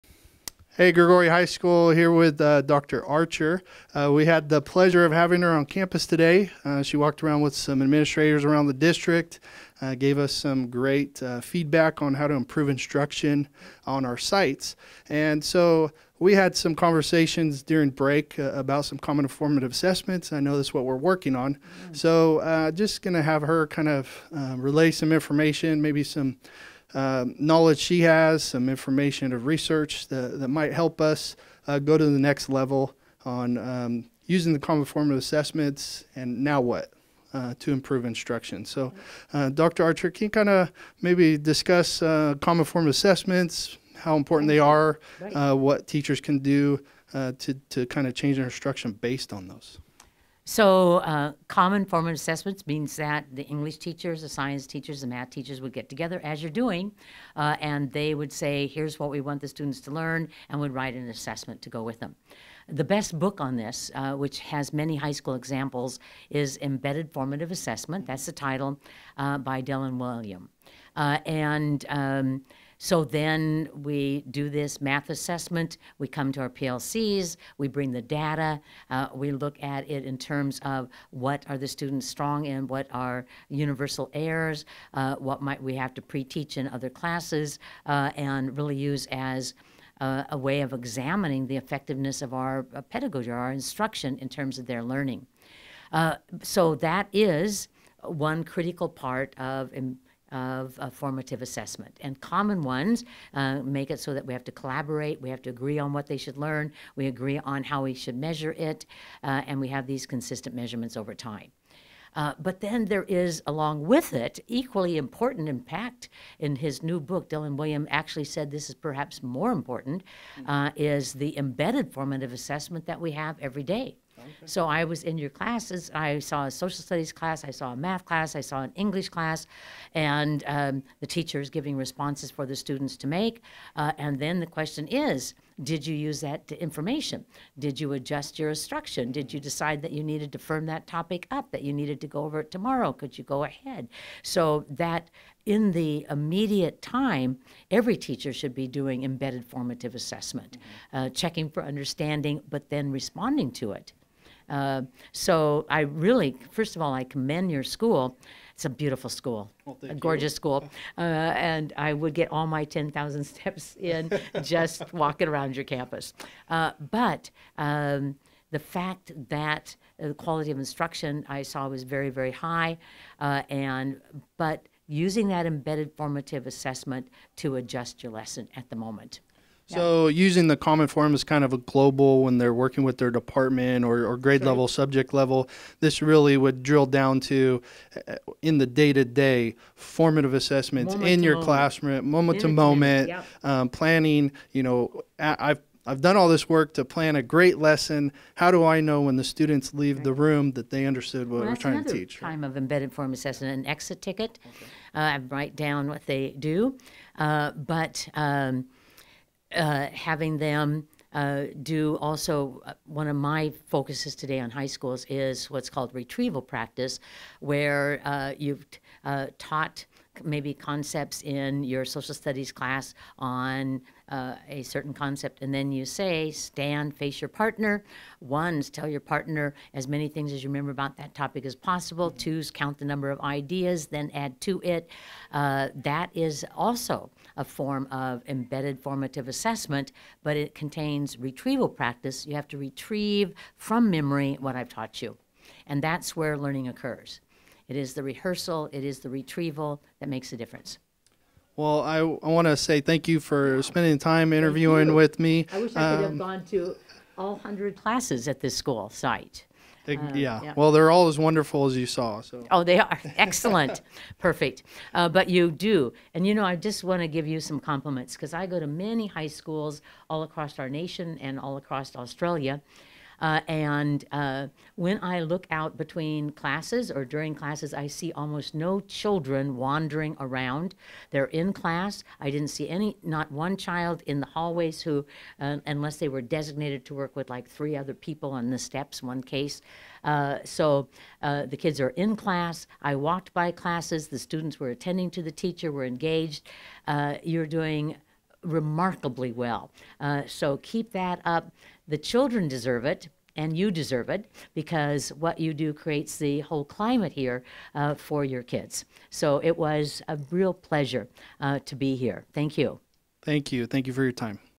Instructional Corner - Dr Anita Archer
Interview with Dr Anita Archer on Feb. 28, 2019 around instructional practices specifically addressing Common Formative Assessment